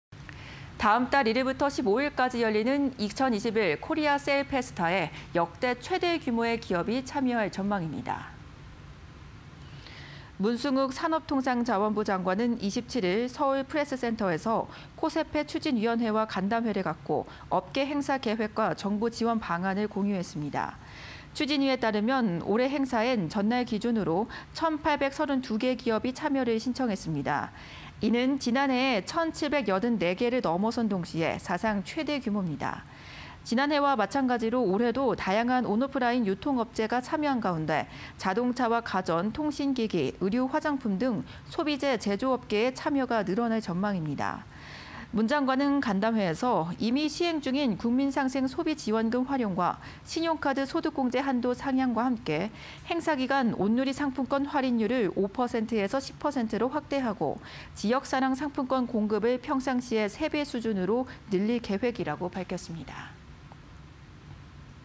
[이데일리N] 코세페... 참여기업 1832개로 역대 최다 (20211028)
다음 달 1일부터 15일까지 열리는 '2021 코리아세일페스타'(이하 코세페)에 역대 최대 규모의 기업이 참여할 전망입니다.
문승욱 산업통상자원부 장관은 27일 서울 프레스센터에서 코세페 추진위원회와 간담회를 갖고 업계 행사 계획과 정부 지원방안을 공유했습니다.
추진위에 따르면 올해 행사에는 전날 기준으로 1천832개 기업이 참여를 신청했습니다. 이는 지난해의 1천784개를 넘어선 동시에 사상 최대 규모입니다.
지난해와 마찬가지로 올해도 다양한 온·오프라인 유통업체가 참여하는 가운데 자동차, 가전, 통신기기, 의류·화장품 등 소비재 제조업계의 참여가 늘어날 전망입니다.
문 장관은 간담회에서 이미 시행 중인 국민상생소비지원금 활용, 신용카드 소득공제 한도 상향과 함께 행사 기간 온누리 상품권 할인율을 5%에서 10%로 확대하고, 지역사랑상품권 공급을 평상시의 3배 수준으로 늘릴 계획이라고 밝혔습니다.